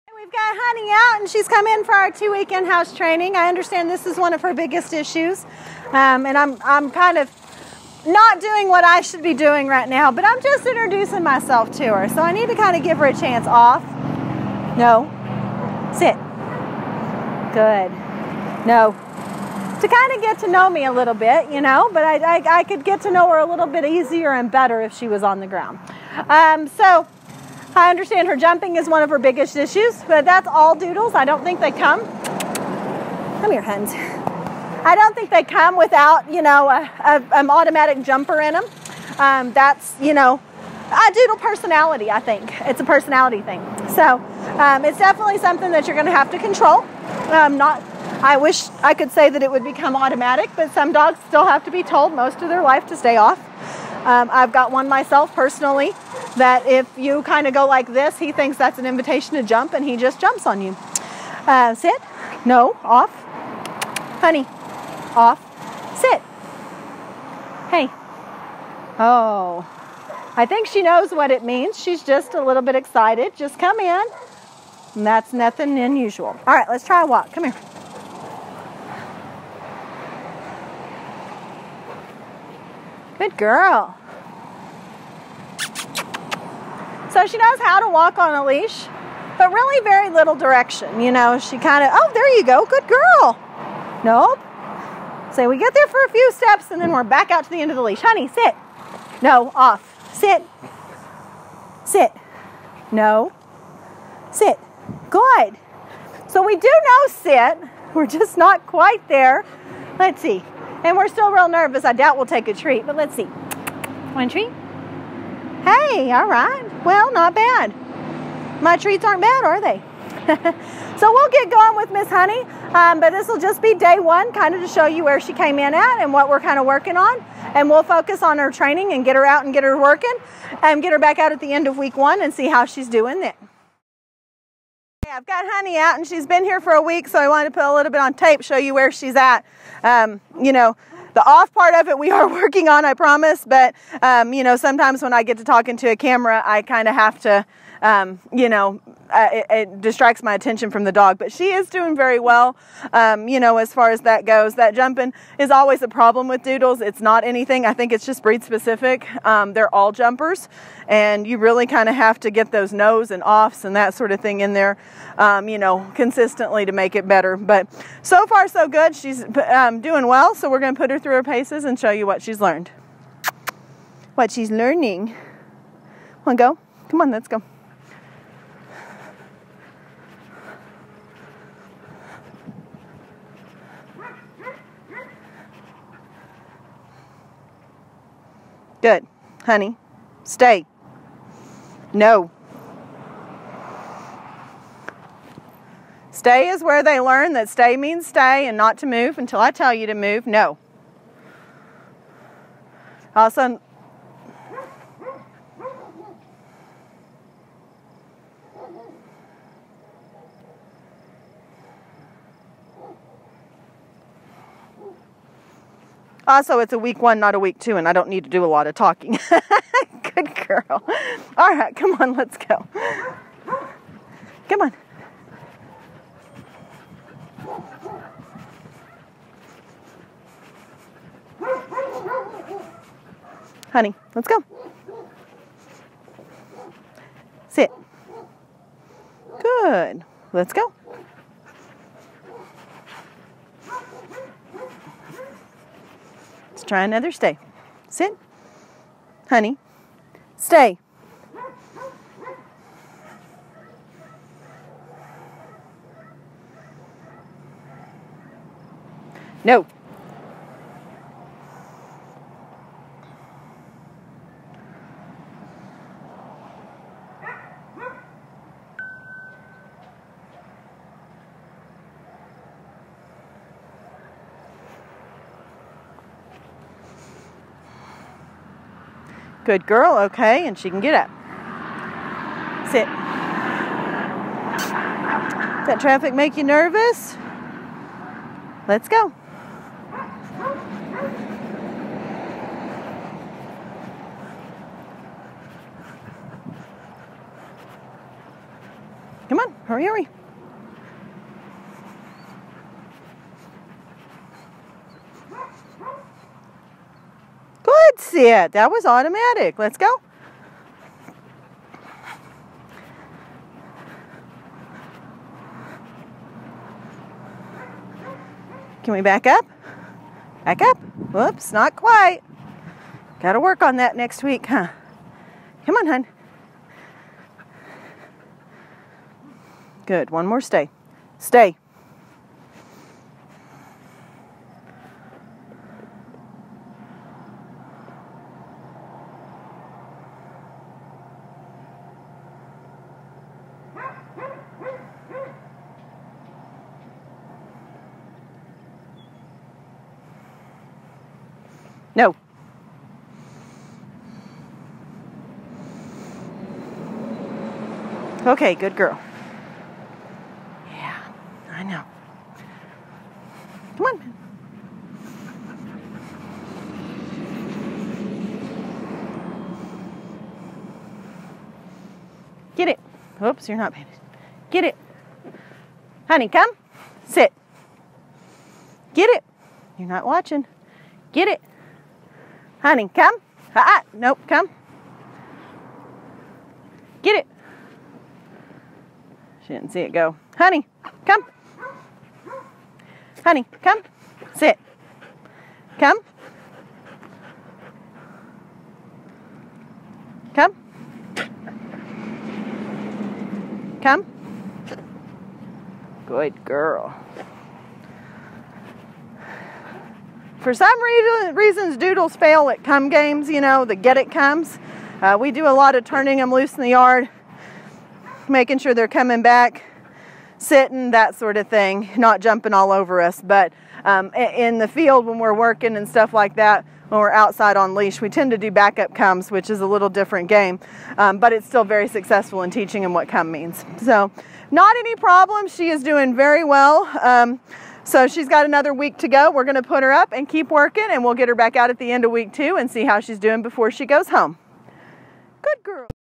Honey Day One/Week One Training
Honey is a sweet girl just a typical doodle.  She loves to be where you are and up as close as she can get.  She's working hard on not jumping and making improvements every day. Here's a new training client after their first week of training.  Everyone will be at different stages but we love to show them off!